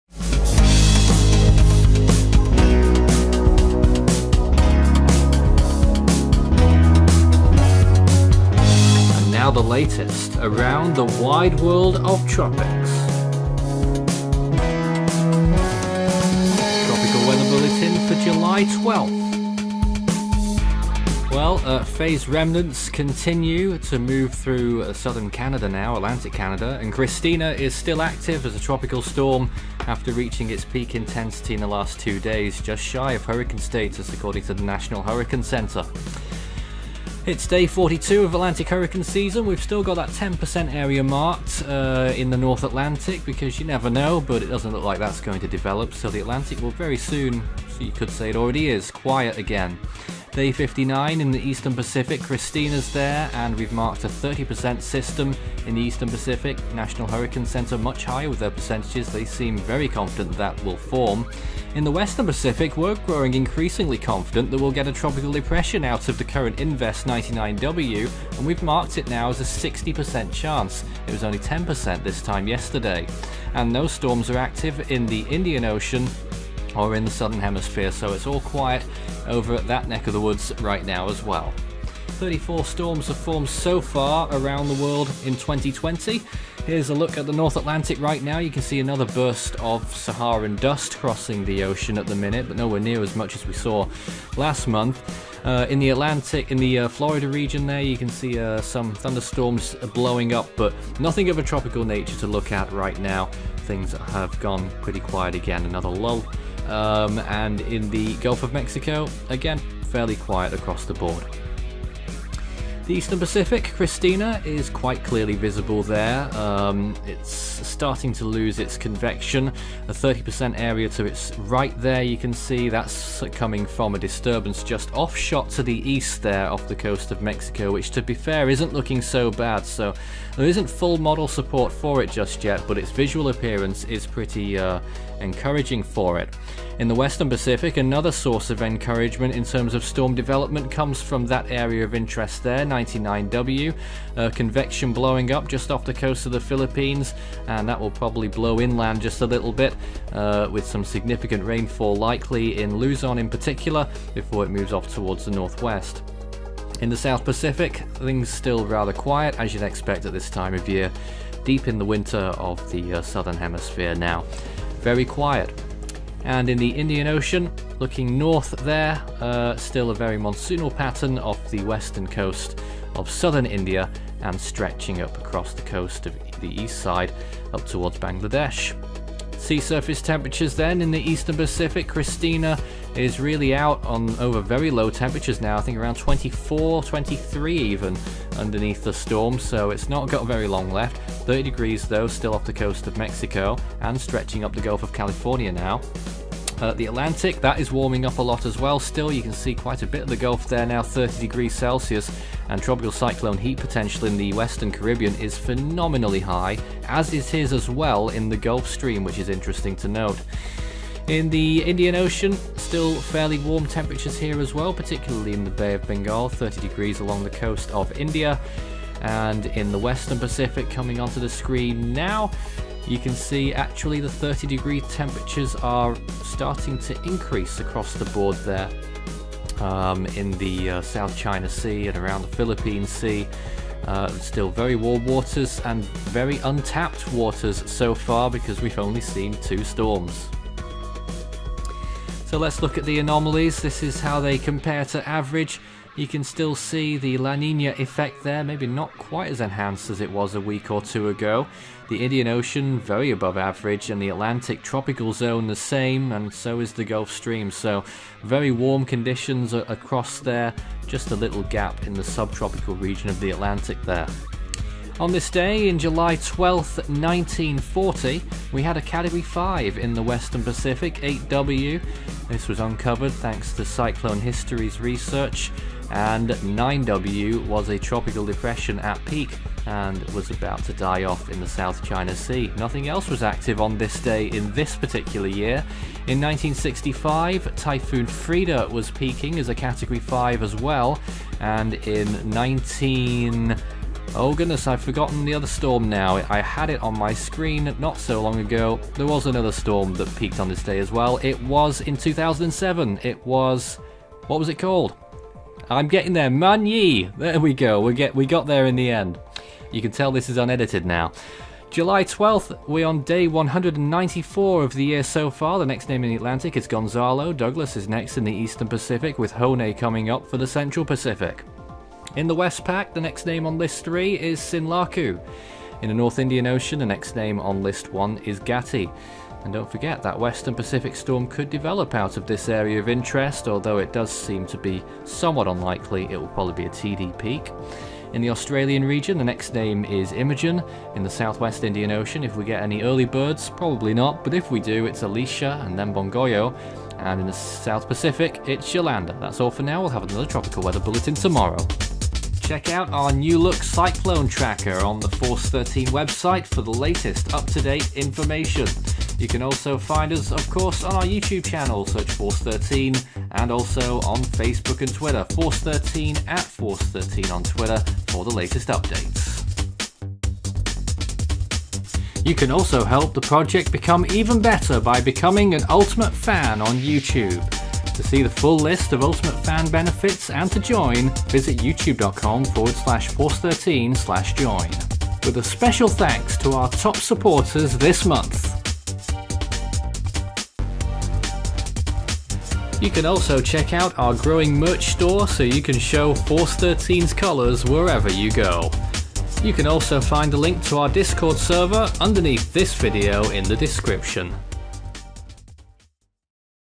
Tropical Weather Bulletin -Tropical Storm Cristina and Ex-Fay - July 12, 2020
The remnants of Tropical Storm Fay have moved into southern Canada and are fairly weak at this point - elsewhere in the Atlantic an area of interest still has a small chance of development several hundred miles west of the Azores.

Tropical Storm Cristina is now weakening with waning convection after yesterday's peak. The storm is likely to weaken more rapidly now over cooler waters, and die off in a couple of days.

In the Western Pacific, an area of interest now has a moderate chance of developing into a tropical depression off the northern Philippines, and could deliver significant rainfall to the area and then onto Taiwan.